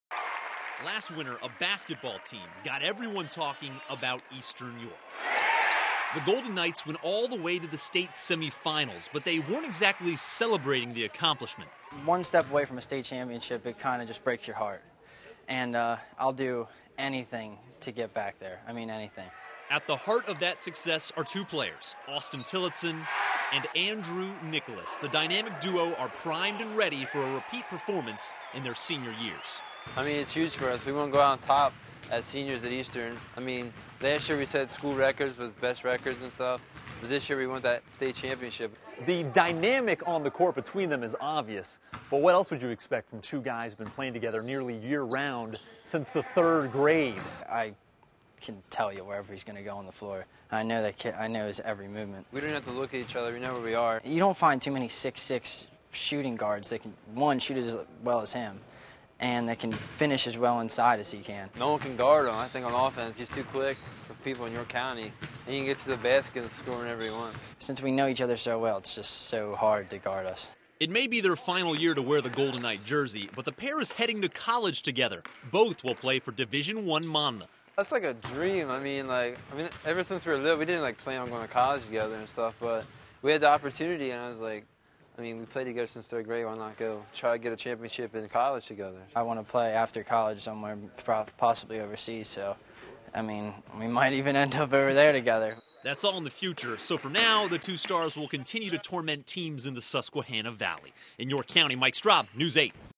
Eastern York's Dynamic Duo Back For Another Round
The Golden Knights went all the way to the state semifinals one year ago, but seniors Austin Tillotson and Andrew Nicholas aren't satisfied and want to go out on top this year.